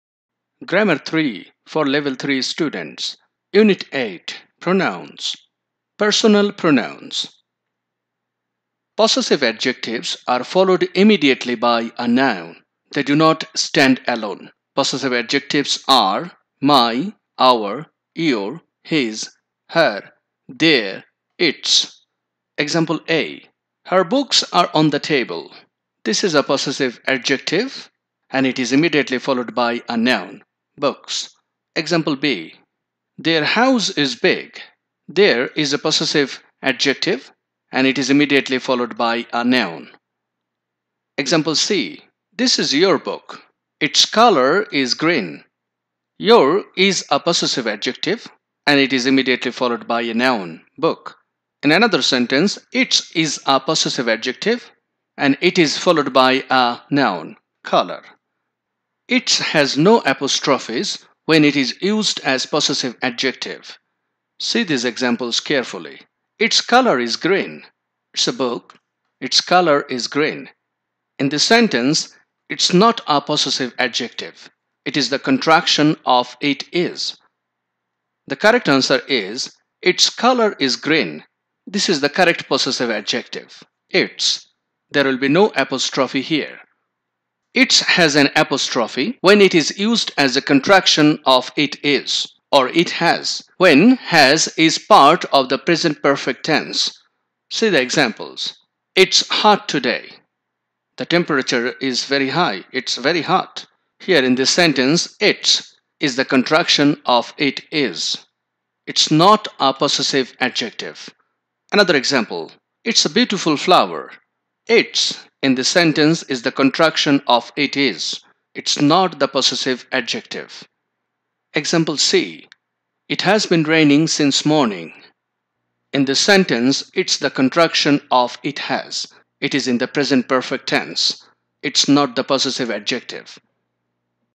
Grammar 3 | ch8 | Personal Pronouns -3
المقررات المفتوحة - Grammar 3
Chapter 8: Personal Pronouns

1- Possessive adjectives followed by nouns
2- Examples
3- Usage of apostrophes with its
4- Examples